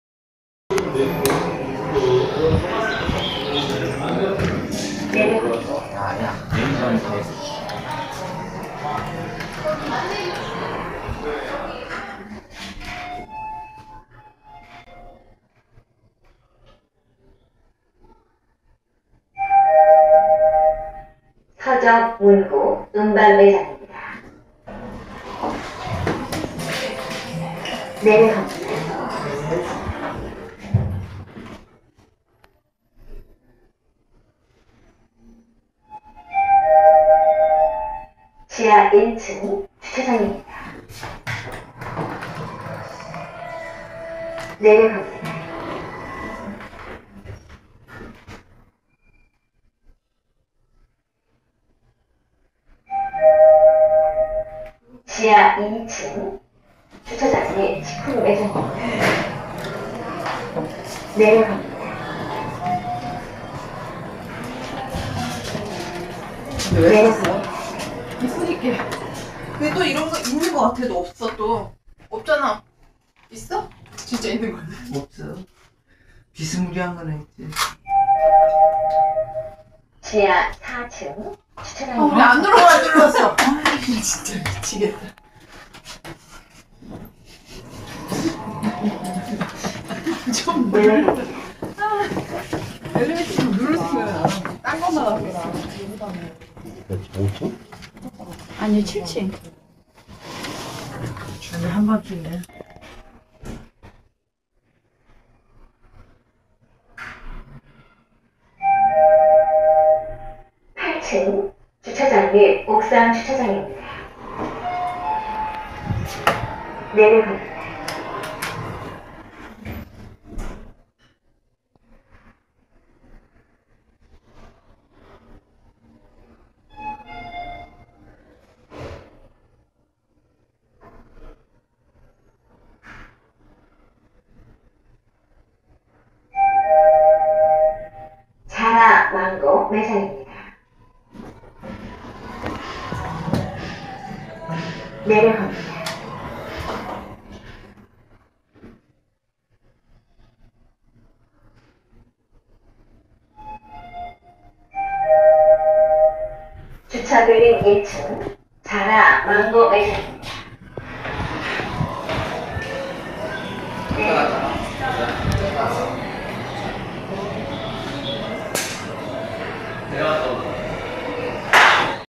(현재 대차) 대전광역시 서구 둔산동 (주)한화갤러리아티임월드 현대엘리베이터 탑사기
시작에 앞서 지하4층~7층까지는 각층마다 이용객이 다량 탑승으로 그 부분은 녹화 되지 않았습니다.  이점 감안하시어 시청해주셨으면 합니다.

2013년 5월 17일 영상분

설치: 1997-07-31
모델: BAX-VF
종류: 승객용

현재는 14년 3월 현대 LXVF-7 로 대차되었습니다.